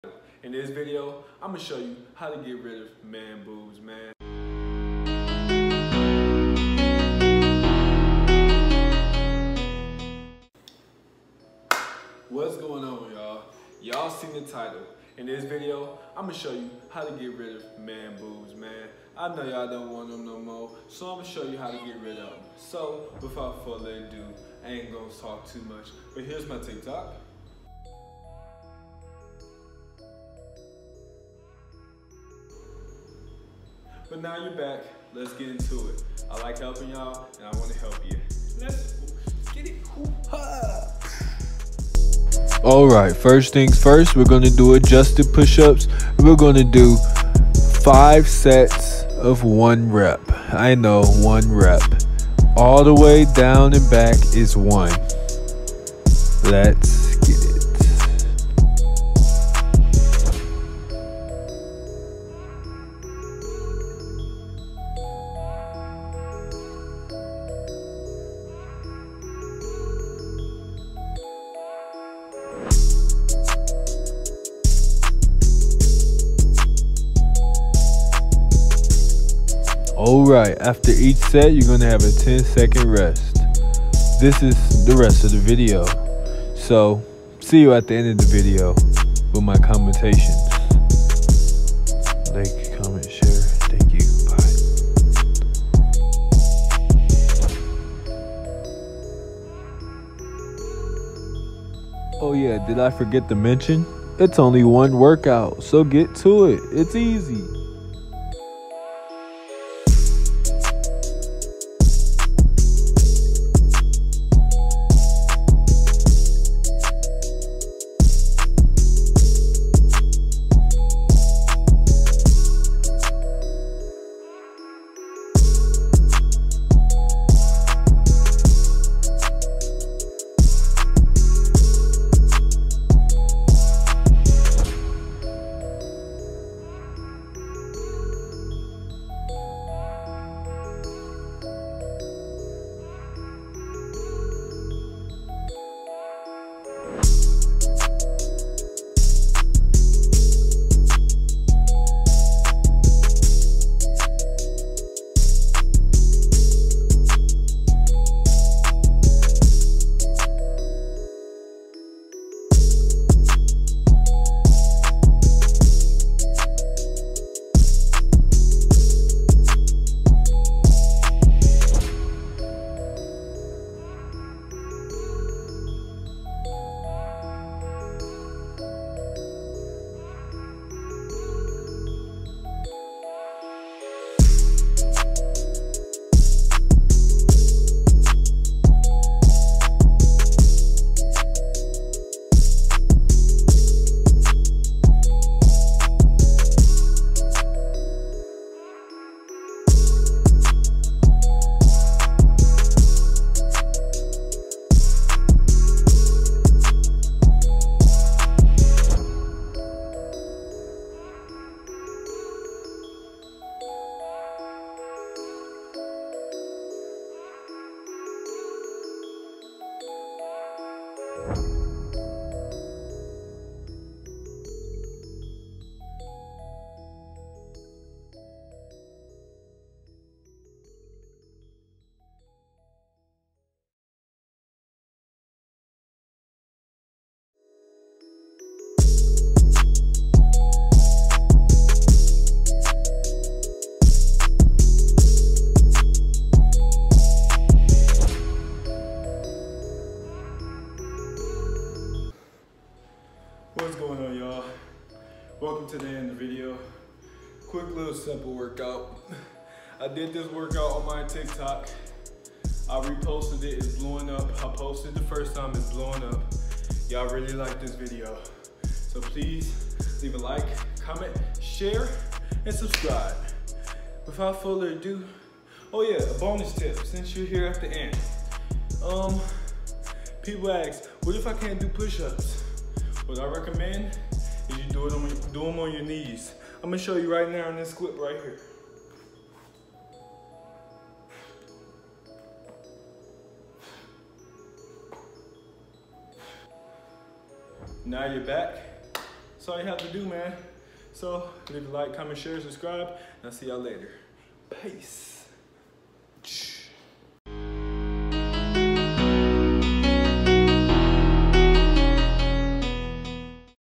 5 MINUTE MAN BOOB WORKOUT! // No Equipment | CrowningOfTheTj
5 minute man boob loss exercise that requires no equipment! Let get it!

Share to any friends who needs to lose weight! Also be sure to check out my diet video down below!

Do this side fat exercise once or twice every single day! Rest Sunday! FOLLOW ALONG and Let’s lose chest fat together!

Getting abs and/or muscle is not that easy. I’ve seen a lot of videos showing how to get Abs in the short amount of time. and to be honest that’s not true nor possible. it’s not gonna happen in the short amount of time. But I’m going to show you is what I’ve been doing and how i obtain a eight pack in my three-year term goal.